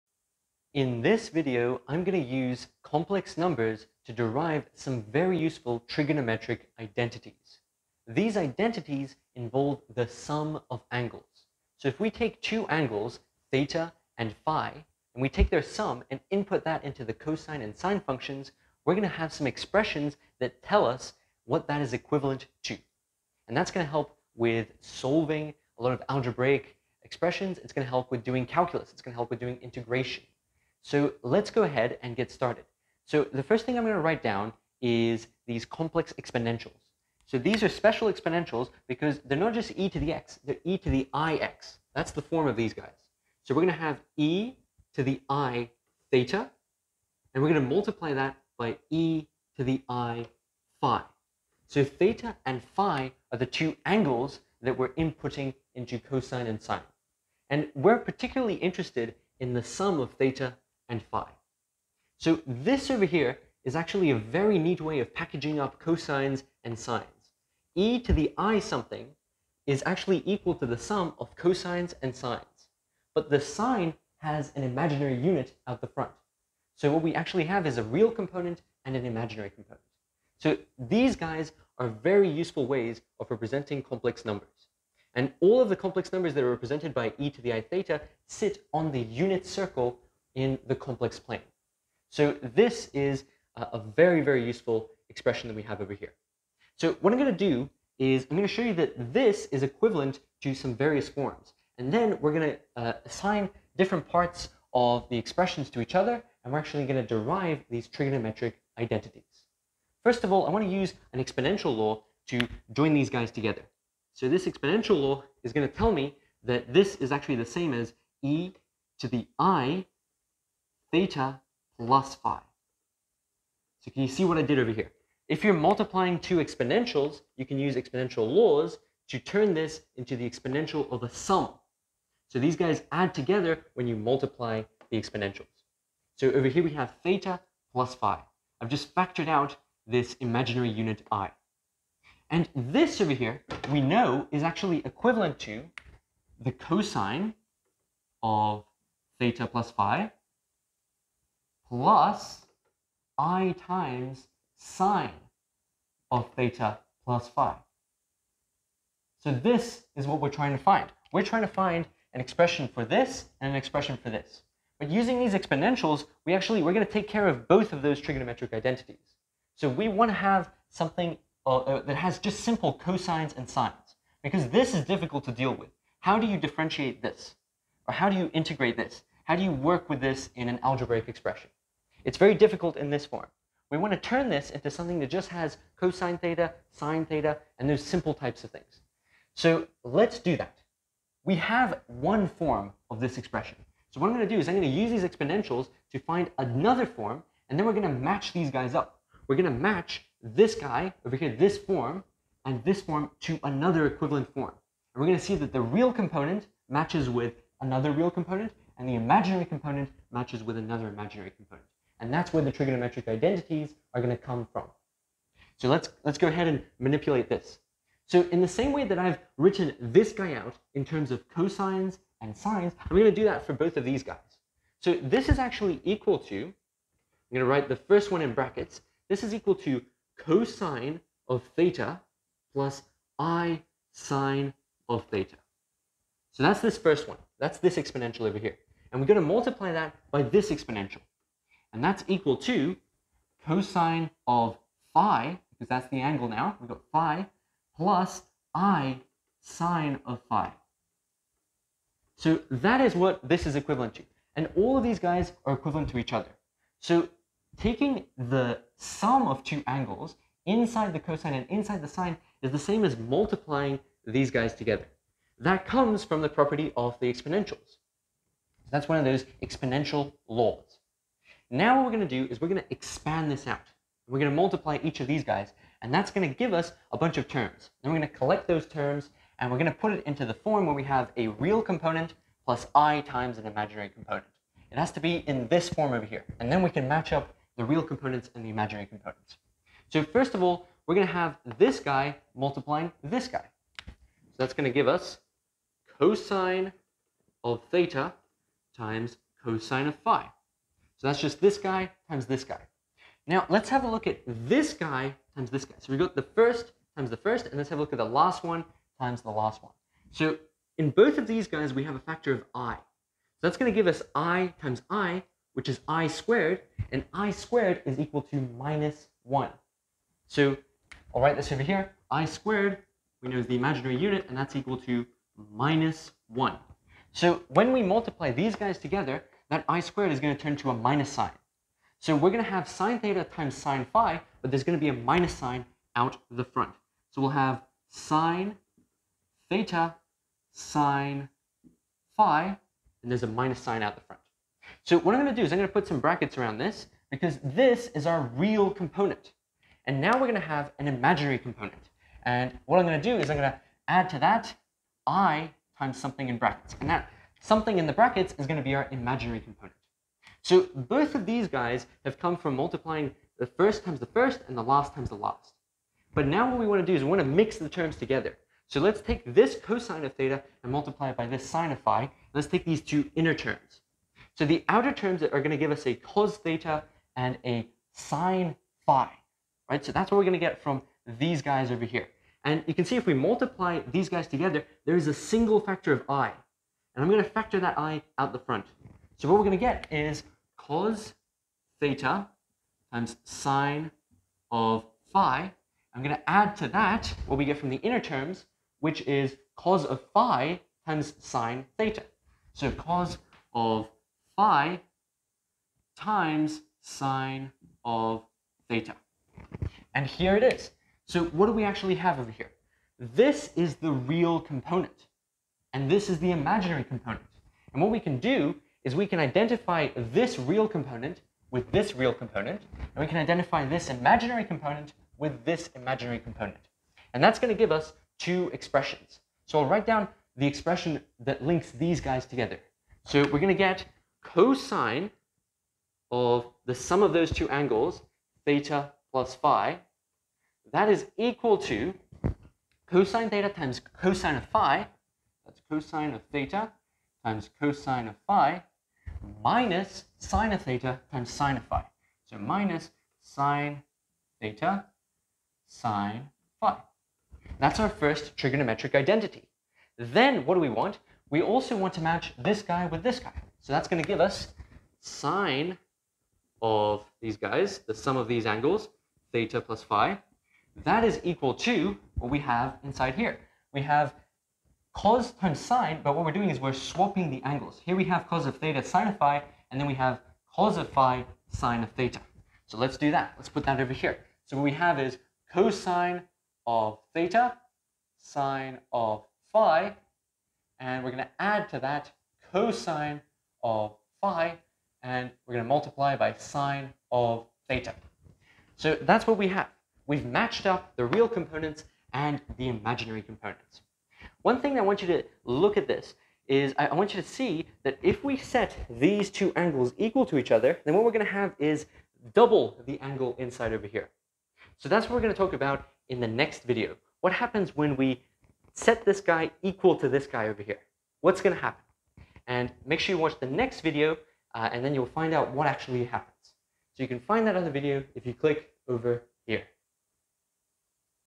Angle Sum Trigonometric Identities Derivation || Complex Exponentials and Euler's Formula
I introduce mathematical concepts which will be used in subsequent classical and quantum mechanics videos in the playlist above.